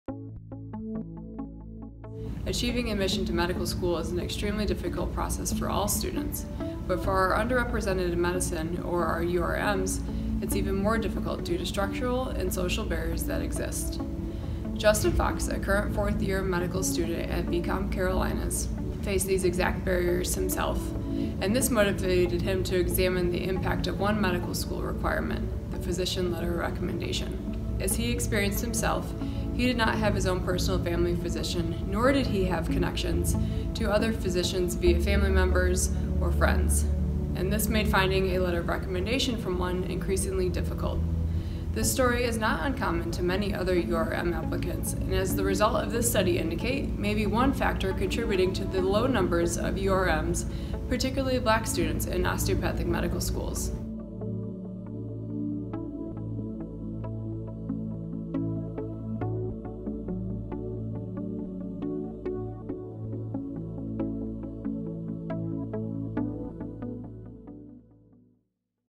JOM Article, Recommendation Letters
In this study from the AOA's Journal of Osteopathic Medicine, researchers dive into the relationship between recommendation letters and the concerning decrease in diversity within the medical field.